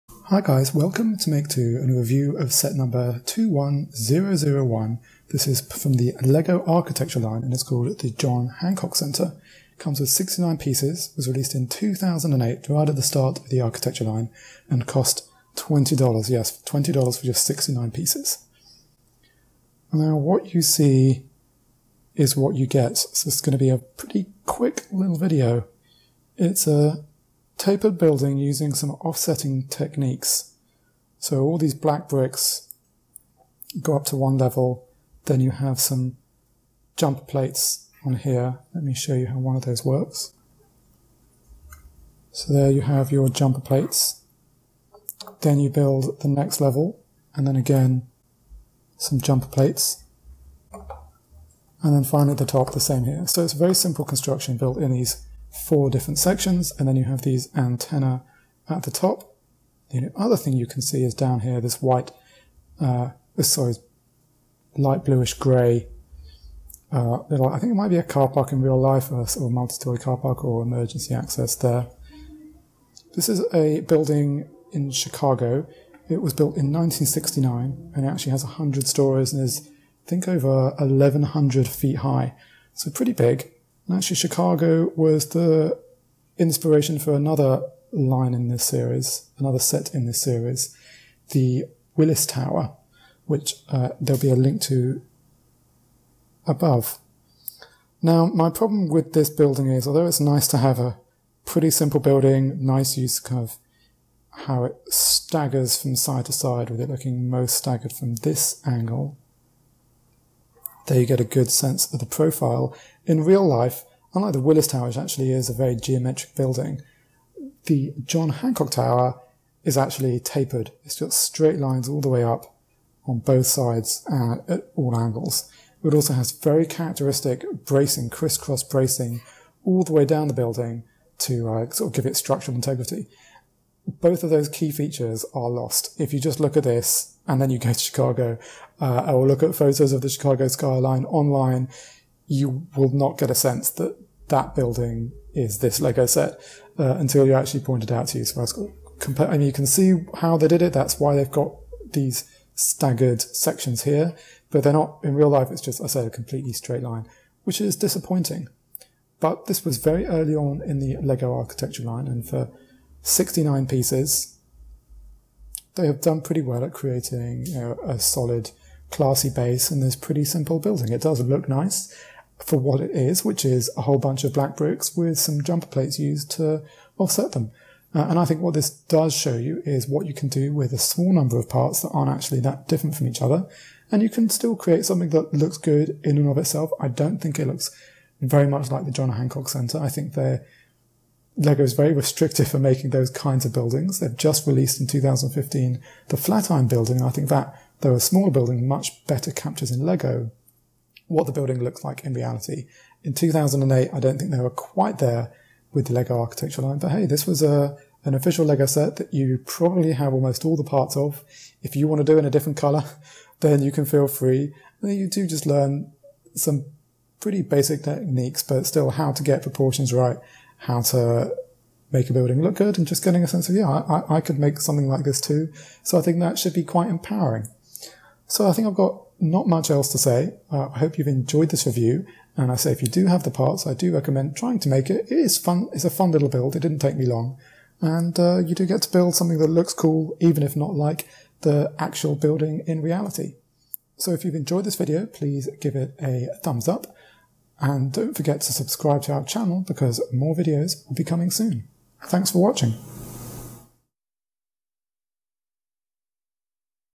LEGO Architecture John Hancock Center Review! LEGO 21001
The early LEGO Architecture sets seem to be more proof of concept rather than excellent sets in their own right. The LEGO John Hancock Center, a very early one, feels in keeping with the line but just a sad miss. This shows you what you can do with 65 pieces but looks so little like the actual building in Chicago, in which it was based, that I am left scratching my head. Still, it should be inspiring to us amateurs that we could make a sense at least as good ourselves as a real LEGO set.

Check our channel for other building set reviews and opinions.